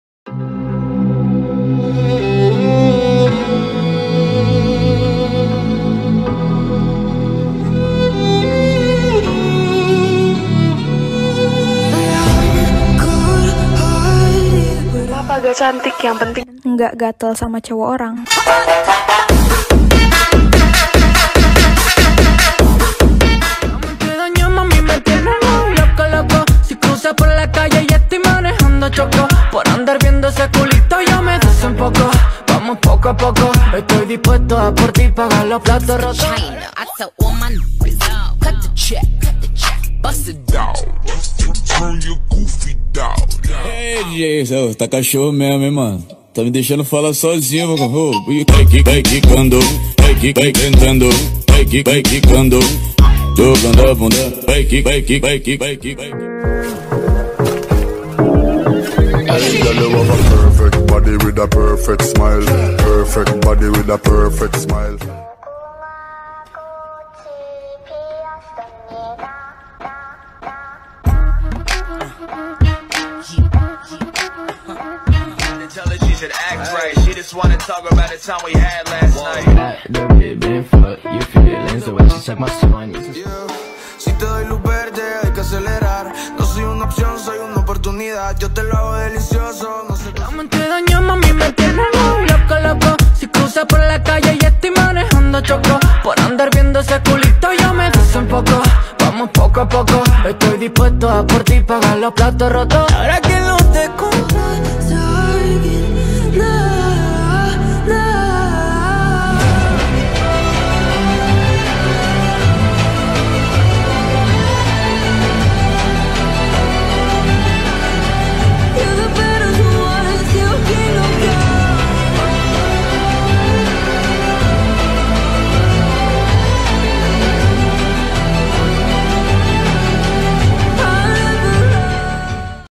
Tiktok Battle #18, Indonesia vs Bolivia
We post some of the best tiktok girls. In this videos, Mima from Indonesia and Rissy from Bolivia. Please comment who do you like the most. :)